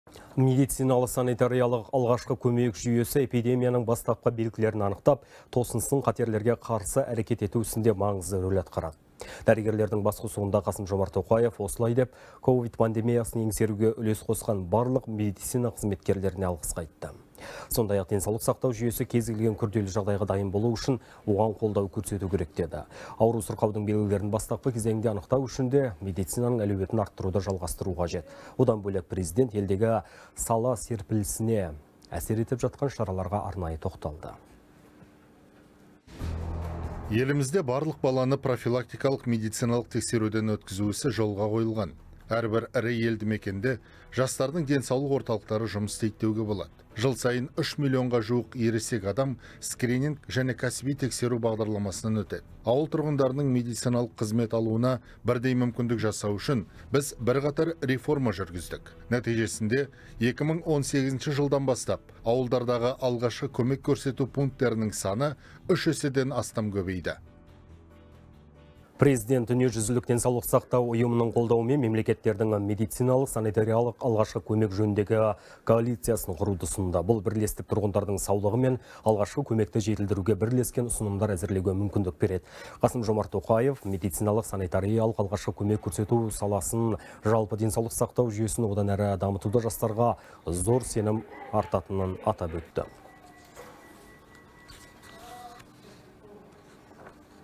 Президент медициналық сақтандыру жүйесінің маңызына тоқталды
Медициналық-санитариялық алғашқы көмек жүйесі эпидемияның алғашқы белгілерін анықтап, тосын сын-қатерлерге қарсы әрекет ету ісінде маңызды рөл атқарады.